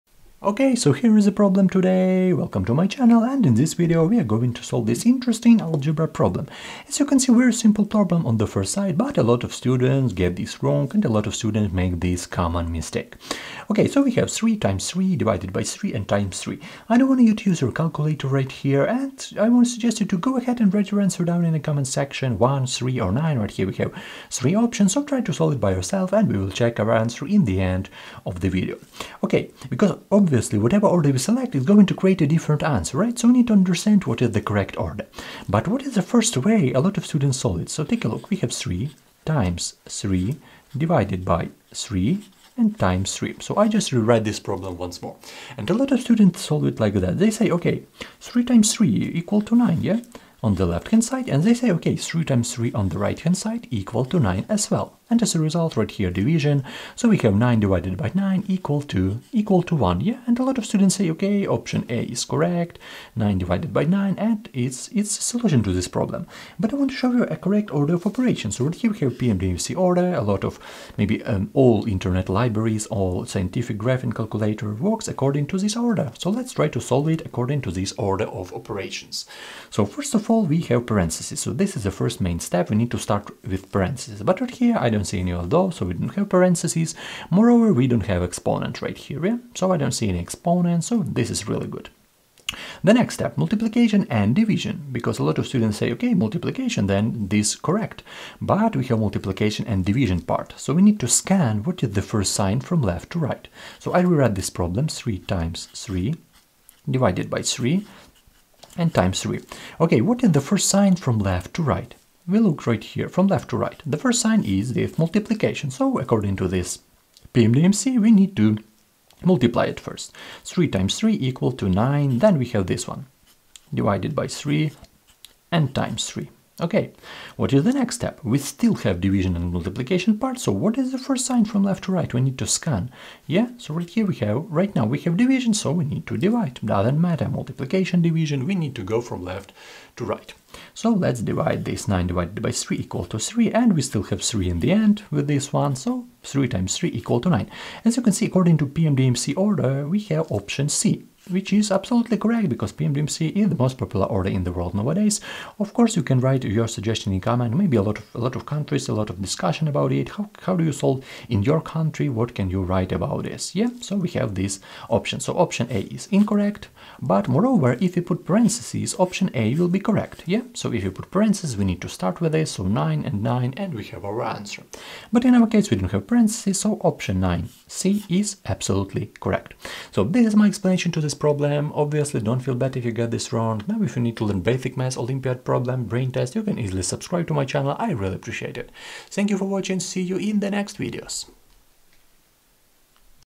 This SAT Question Fools EVERYONE!!
This trick is just the best .You should learn it!!

Hello. In this video, i am going to show you a great Algebra question that you can use to practice your math skills. On this channel, you will get best math problems, questions and solutions to improve your math skills. Math for fun.